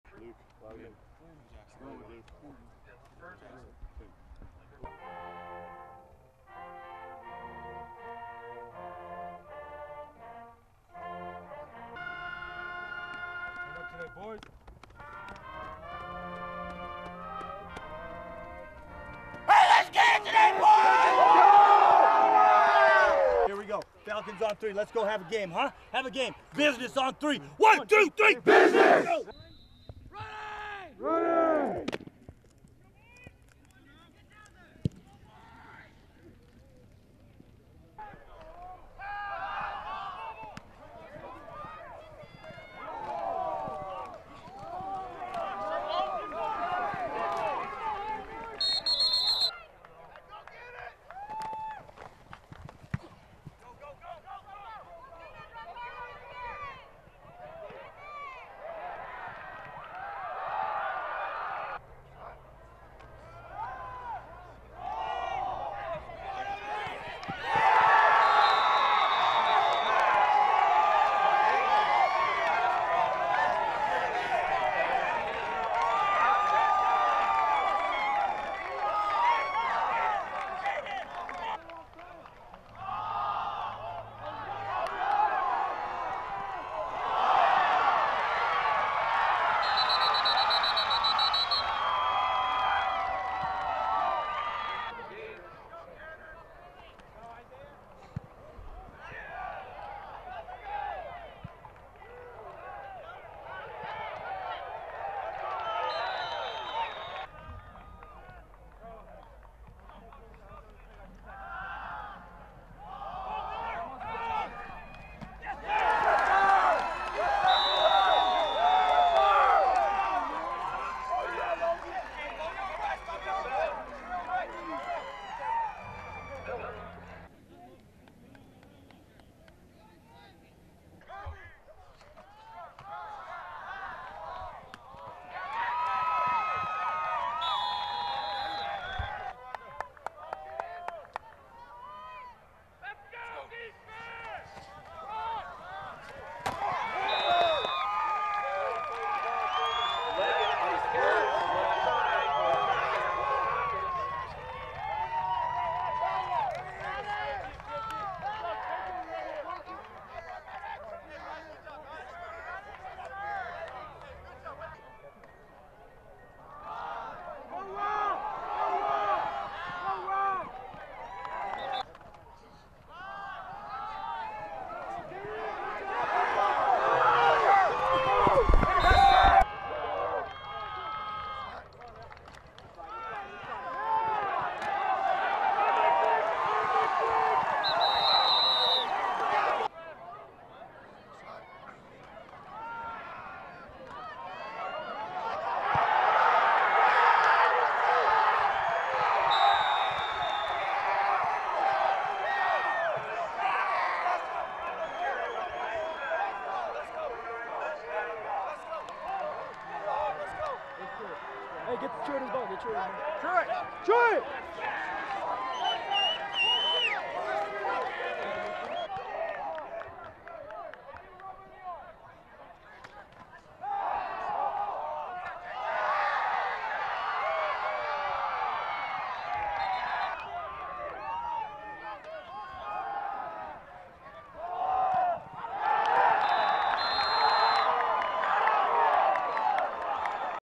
WATCH: Skyline defeats Standley Lake in a close contest
It came down to a two-point conversion!